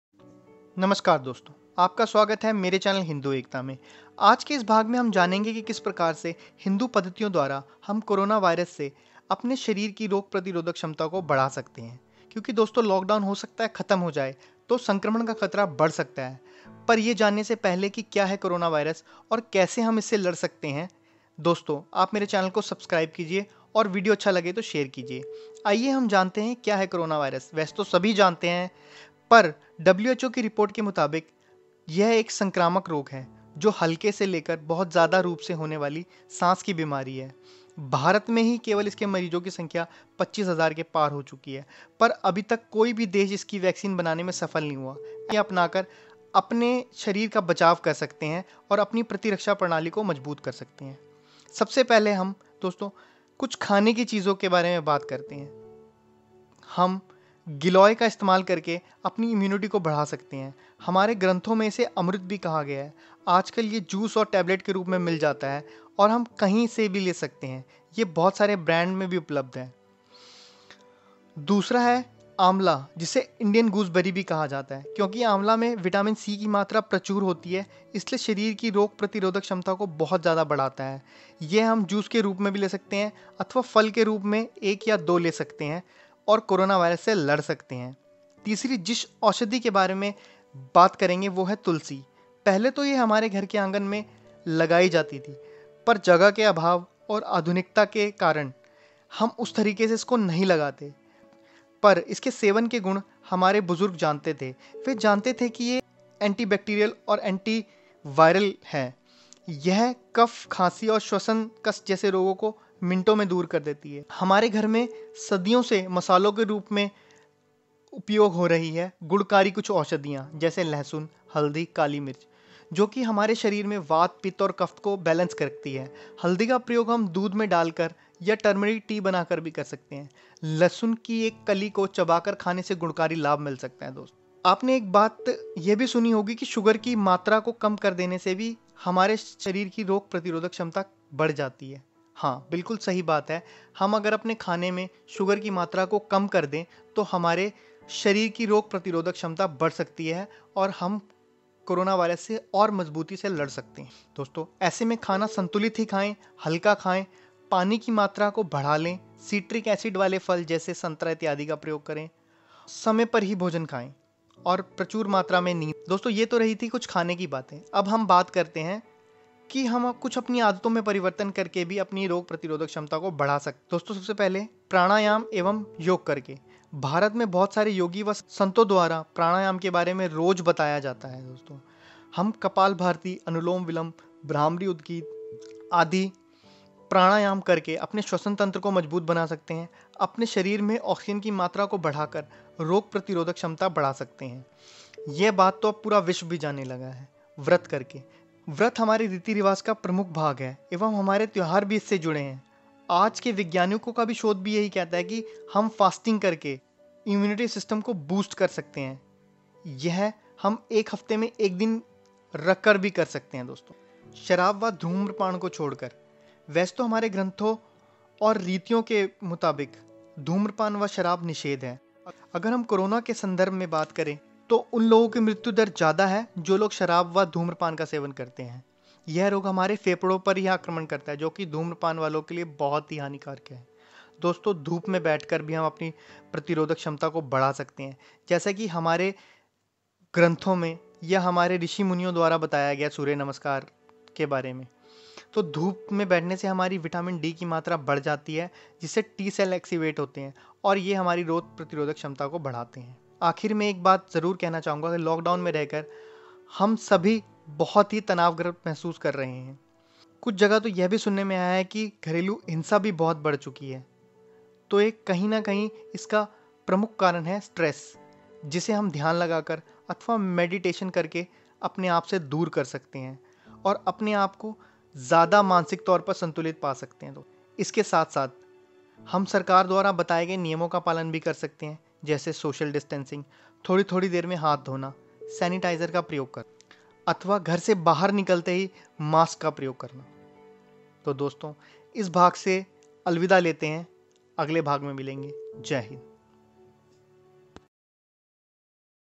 coronavirus se kaise bache hindu paddhati ko apna kar. How to protect yourself from coronavirus
Kuchh hindu traditional niyamo ko apnakar apni immunity ko boost kiya ja sakta hai aur corona virus lada ja sakta hai.